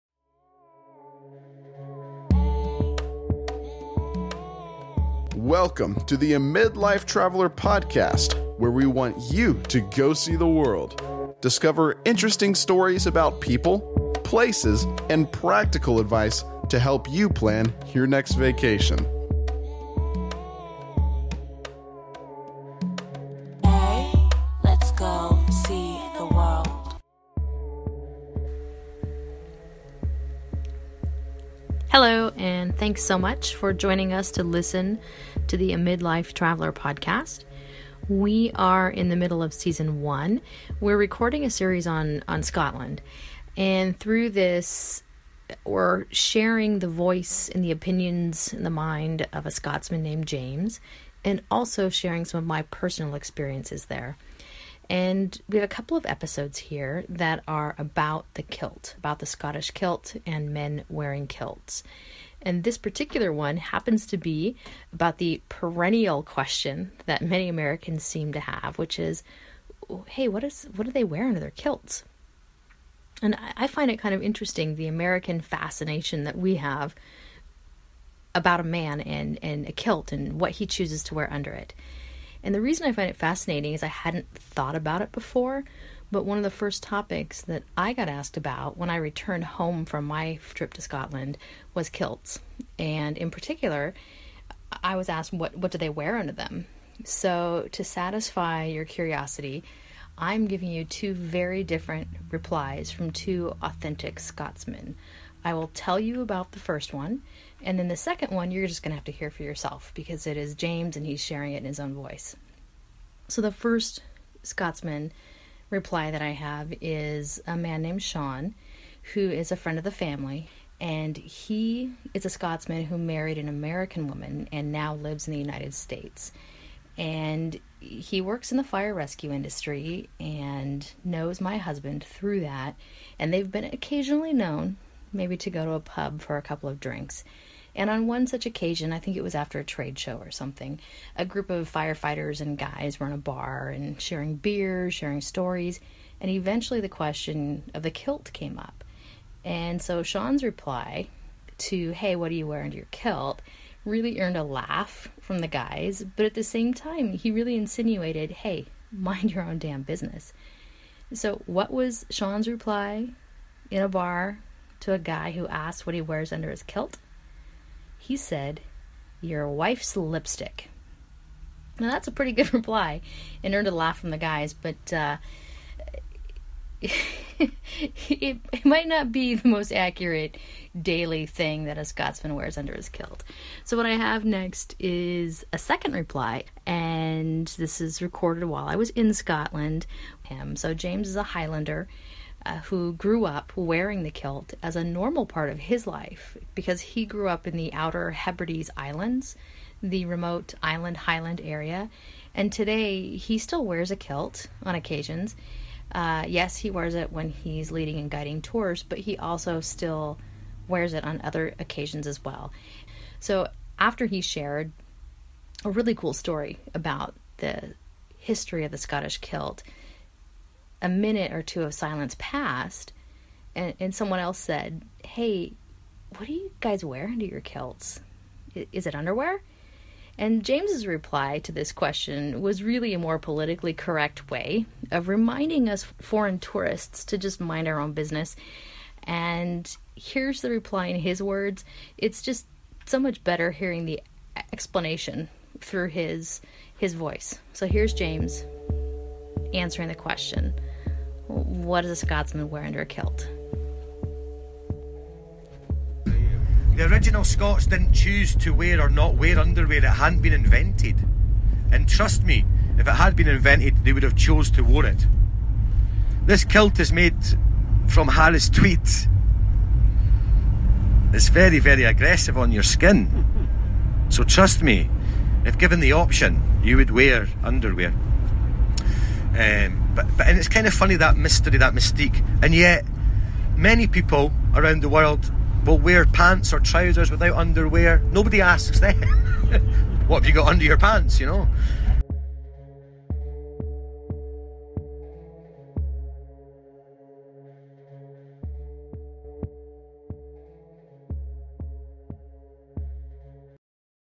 Travel Podcast: What does a Scotsman Wear Under His Kilt?
What's the fascination about a man in choosing to wear a kilt. One of the first topics the podcast host was asked about when I returned home from a trip to Scotland was kilts. And in particular, what do they wear under them? Here to satisfy your curiosity are two very different replies from two authentic Scotsmen.